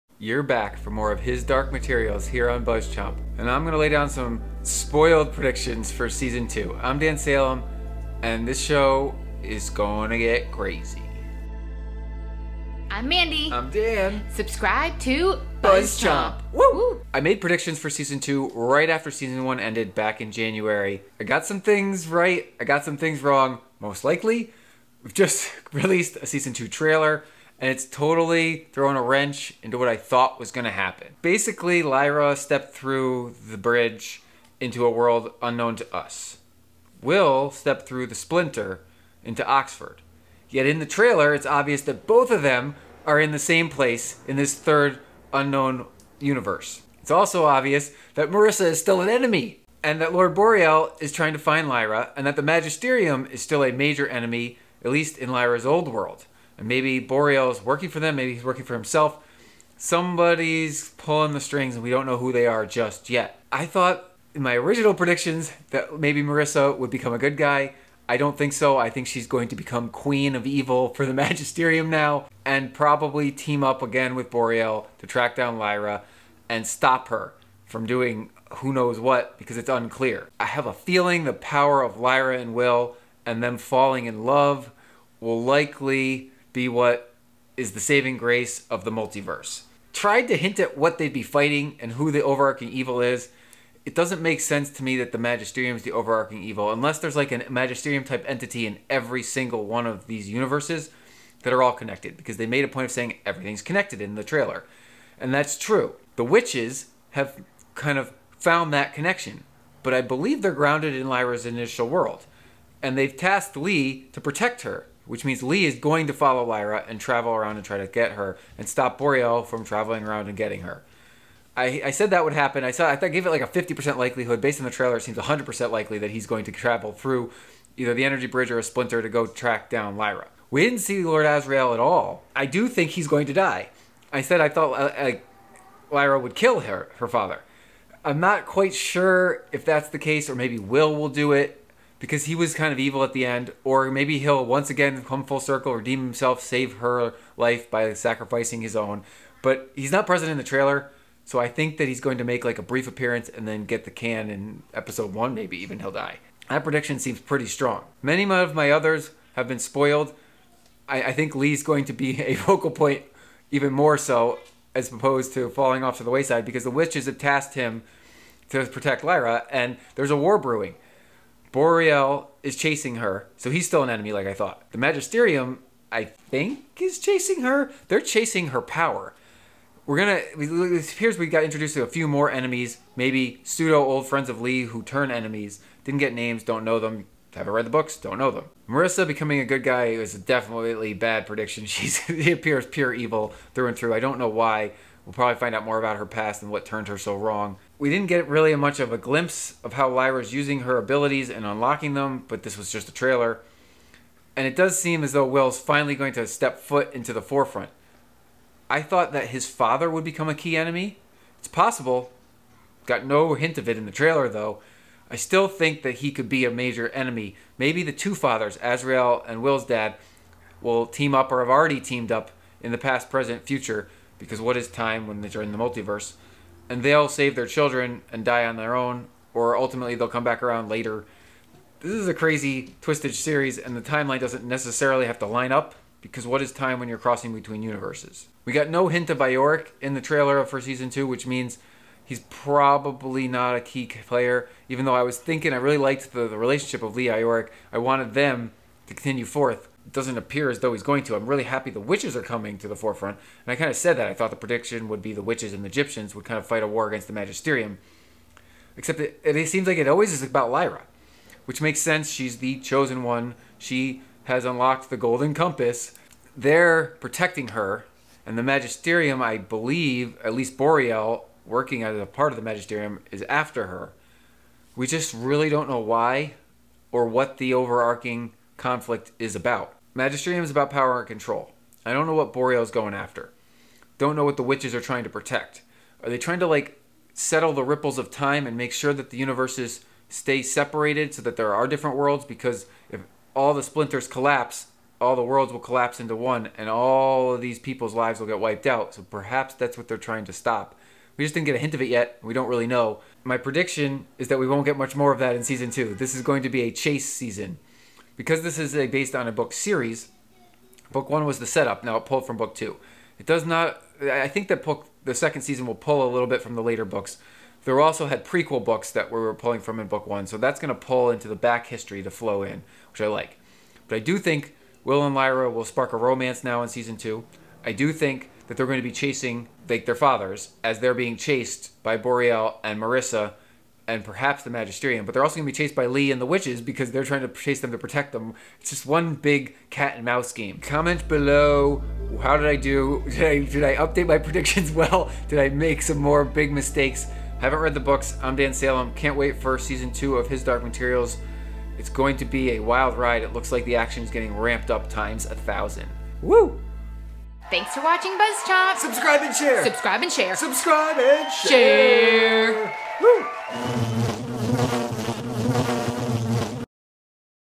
His Dark Materials Season 2 New Predictions from Trailer | BuzzChomp
Having not read the books, these are my bold and new predictions for His Dark Materials Season 2. The trailer gave us way too much to ponder and spoiled a ton of my predictions I made after season 1 ended. But I'm sticking to my big ones. Lyra will kill her father, but I think Will kills his dad now too! They form a lover's bond and track down their dads to the death.

Marisa appears evil through and through, a change from my earlier His Dark Materials Season 2 predictions. I also now believe Lee is front and center in protecting Lyra from whatever evil is plaguing the multiverse. As part of the larger series, season 2 is going to be solely about the chase. We got the set up in season 1, so now Lyra is chasing her dad while being chased by the bad guys. This sets up an epic battle with Will on her side to conclude the series. Am I right? Am I way off? We shall see.